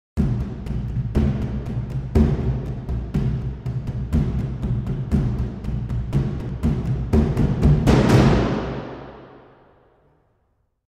Ancient Timpani Beat With Tension
Description: Ancient timpani beat with tension sound effect. Perfect for creating tension, suspense, and excitement.
Artist: Alexander